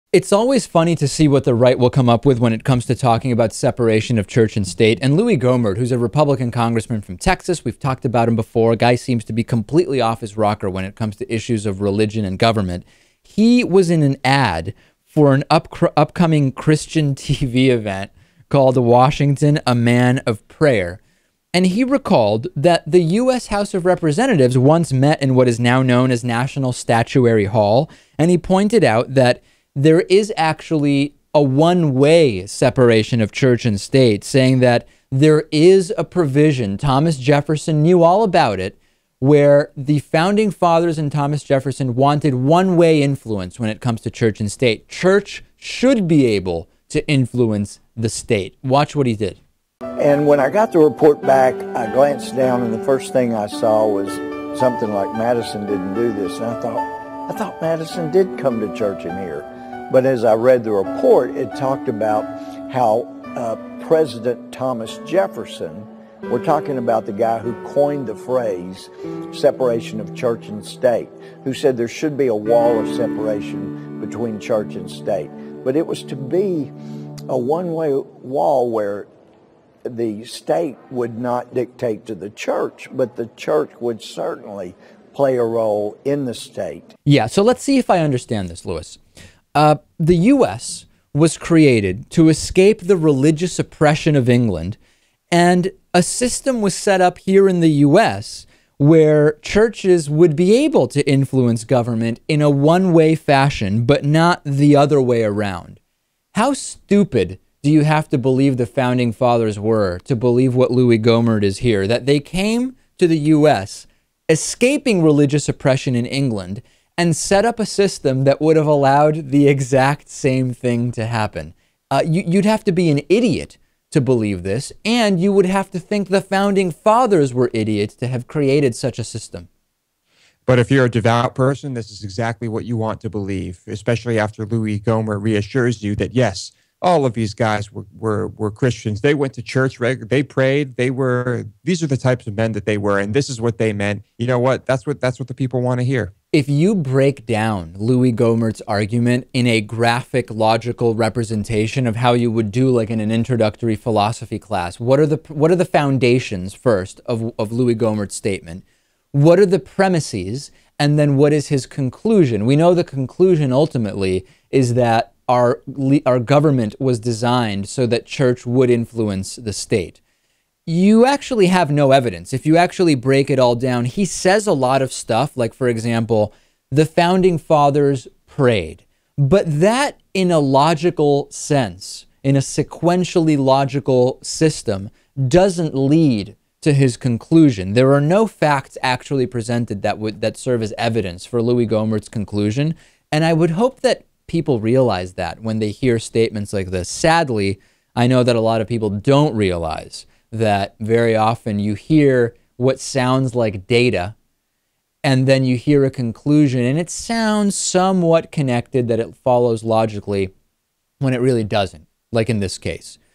Louie Gohmert: 'Separation of Church & State' Means 'Church Plays Role in State'
--Louie Gohmert says that the separation of church and state was meant to be a one-way wall

--On the Bonus Show: Pheromone parties, Facebook and the Oculus, Love Cloud Vegas, more...

24/7 Voicemail Line: (219)-2DAVIDP